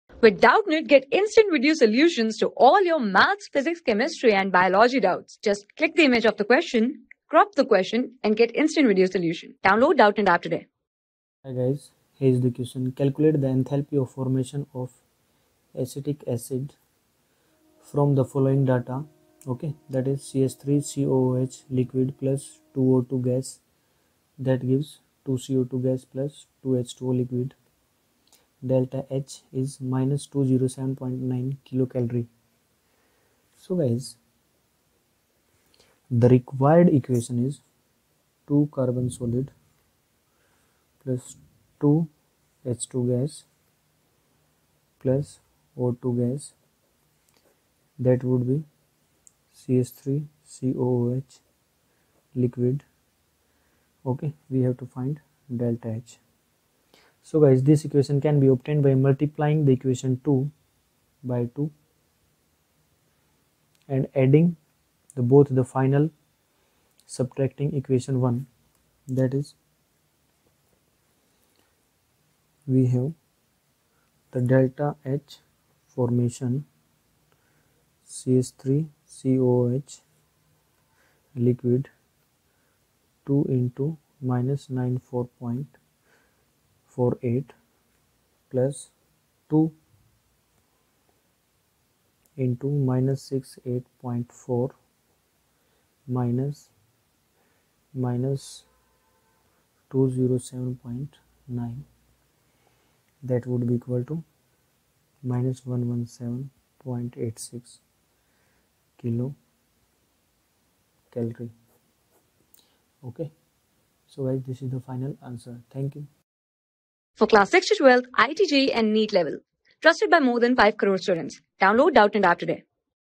Calculate the enthalpy of formation of acetic acid from the following data. CH_(3)COOH(l)+2O_(2)...
Calculate the enthalpy of formation of acetic acid from the following data. CH_(3)COOH(l)+2O_(2)(g)rarr 2CO_(2)(g)+2H_(2)O(l), Delta H=-207.9 k cal
Class: 12
Subject: CHEMISTRY
Chapter: CHEMICAL THERMODYNAMICS
Board:IIT JEE

👉 Have Any Query? Ask Us.